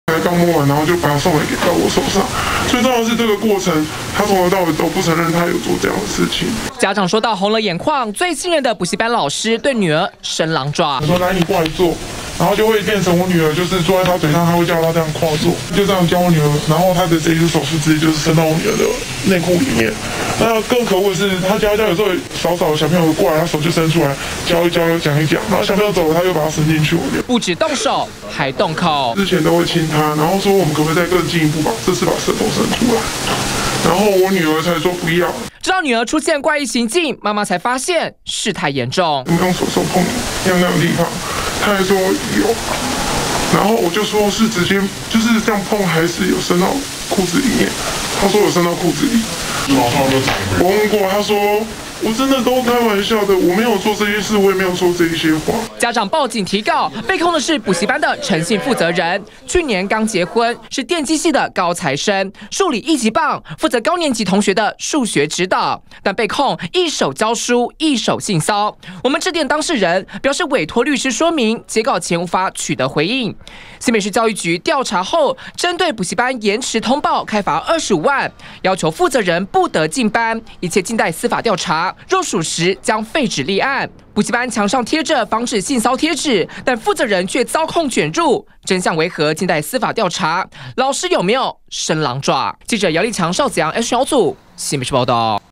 20190401中天新聞 右手教課左手猥褻？ 補教師遭控侵犯小六童
新北市一間補習班的負責人，遭家長指控，趁著教學，要求女童坐在大腿，一手教書，另一隻手居然伸進內褲裡觸碰私處，而且不只一次，誇張的時強迫女童接吻，還要求伸舌頭，家長得知後氣得提告，被控的補習班負責人，否認有性騷行為，還強調是開玩笑，新北市教育局對業者延遲通報，重罰25萬，在司法調查結果出爐前，要求負責人不得進入補習班。